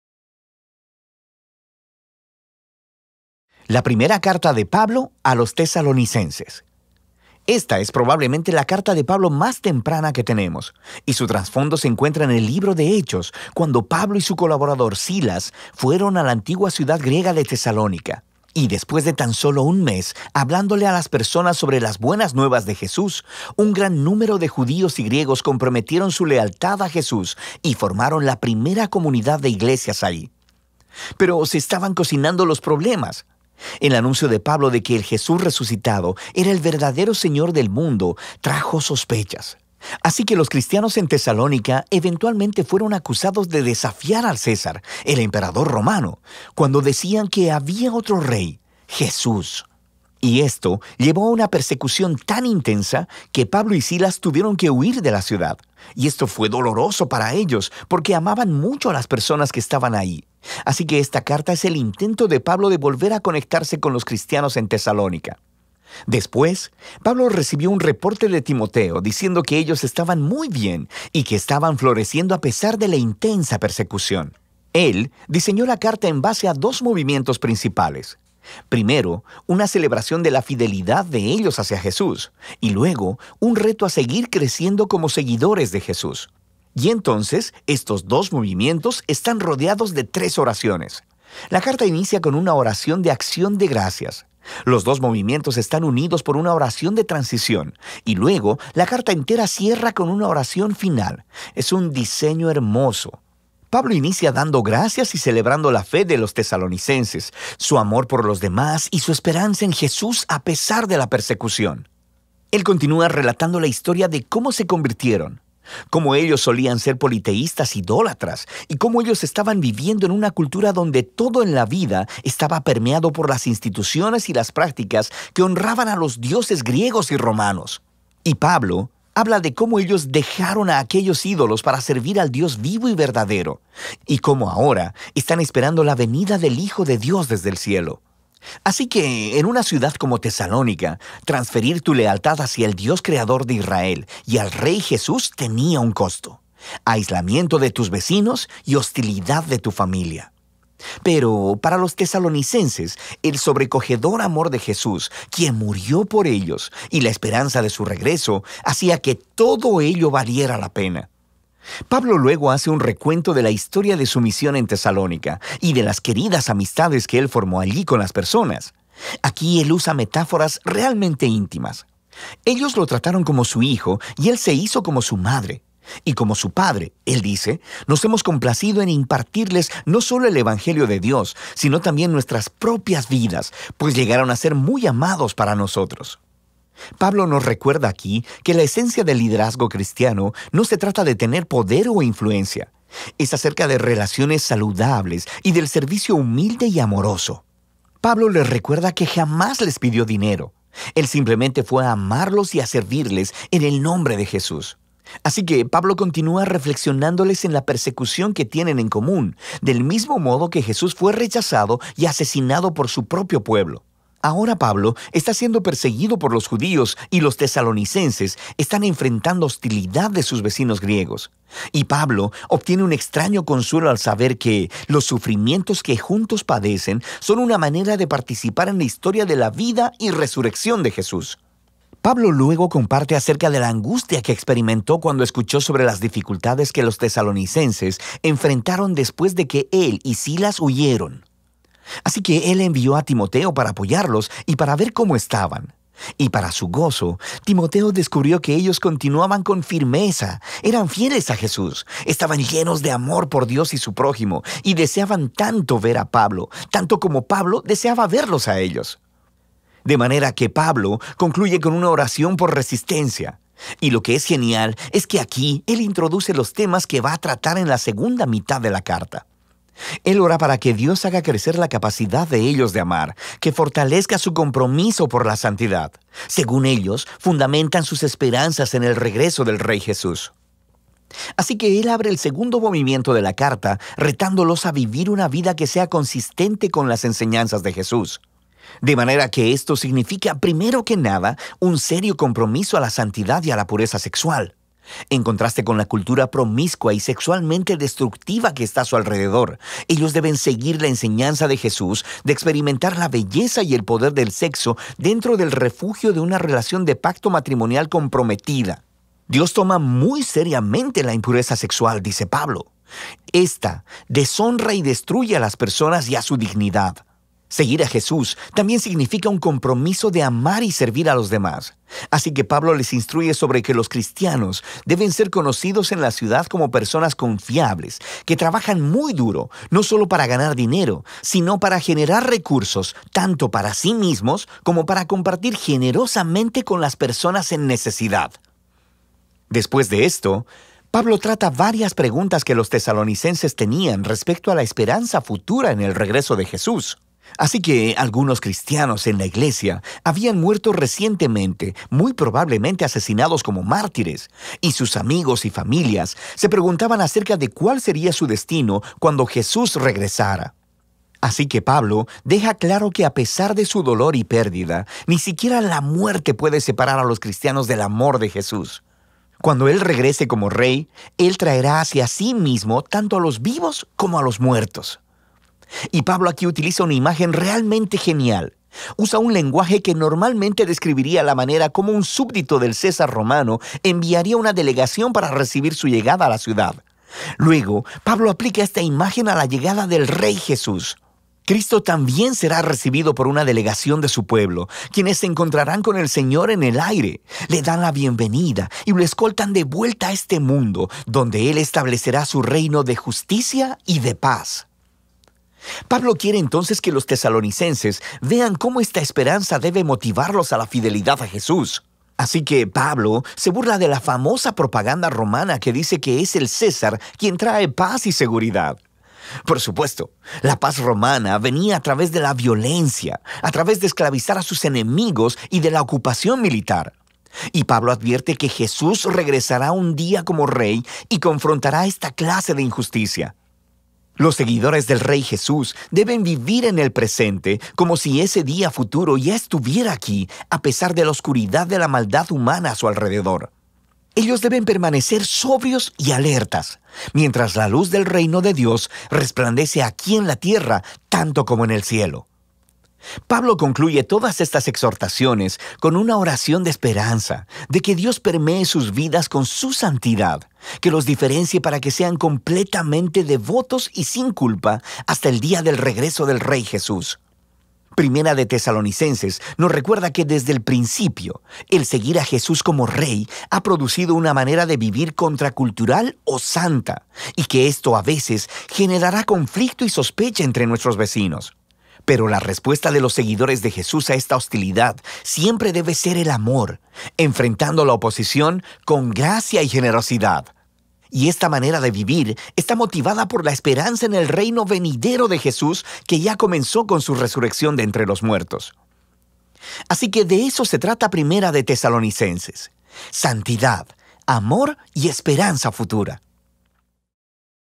Lee la Biblia: 1 Tesalonicenses
El Proyecto Biblia - The Bible Project

Mira nuestro video de Lee la Biblia sobre el libro de 1 Tesalonicenses, que desglosa el diseño literario del libro y su línea de pensamiento. En 1 Tesalonicenses, Pablo dirige a los perseguidos cristianos tesalonicenses a que pongan su esperanza en el futuro regreso del rey Jesús quien arreglará todas las cosas.